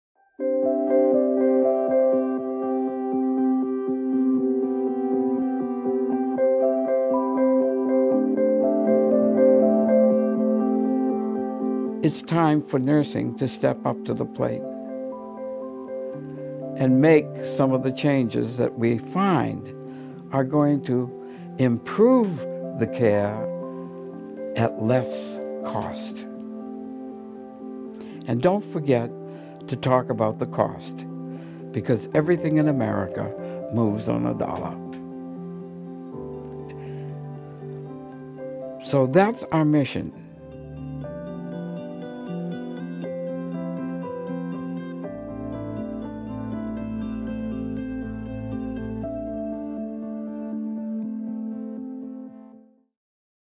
It is Time for Nursing to Step Up
Frontier Nursing University: Distance Education for Nurse-Midwives and Nurse Practitioners

In celebration of National Midwifery Week and the 2016 “Let Our Stories Move You to Answer the Call” Digital Summit, we will be sharing special stories from Kitty Ernst herself. We hope her messages inspire you to continue to Answer the Call as a nurse-midwife or supporter of nurse-midwifery care.